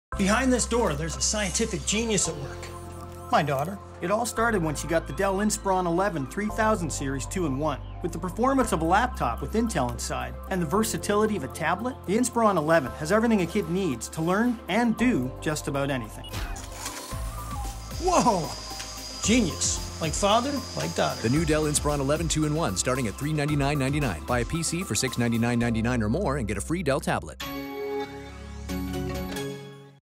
Dell Inspiron Commercial with Actor Les McDonald
I felt fortunate to have been cast as the lead for this Dell Inspiron commercial. Fun fact. The drone was hovering around on fishing line and was not actually flying on it's own!